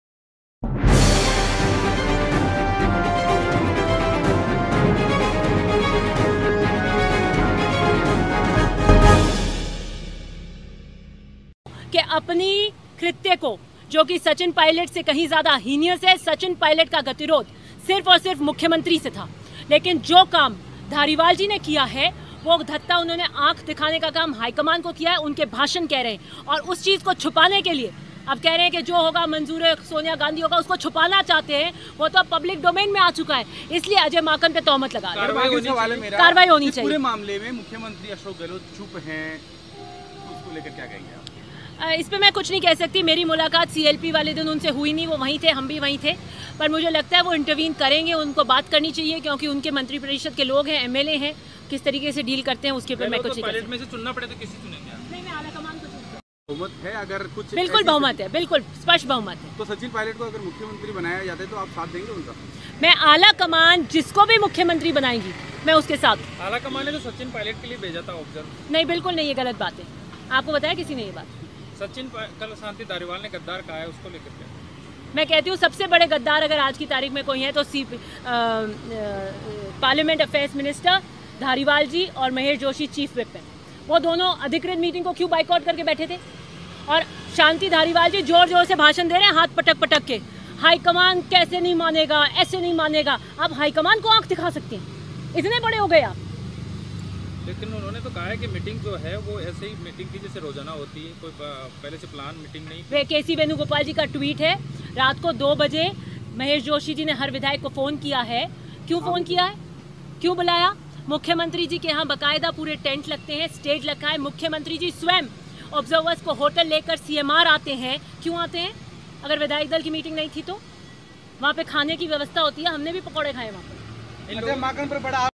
"महेश जोशी और शांति धारीवाल सबसे बड़े गद्दार"; विधायक दिव्या मदेरणा
महेश जोशी और शांति धारीवाल सबसे बड़े गद्दार"
कांग्रेस विधायक दिव्या मदेरणा ने कहा- मेरे लिए आलाकमान सर्वोपरि, महेश जोशी ने ही विधायक दल की बैठक के लिए सब को फोन किया, लेकिन खुद ही बैठक को बायकॉट करते हैं।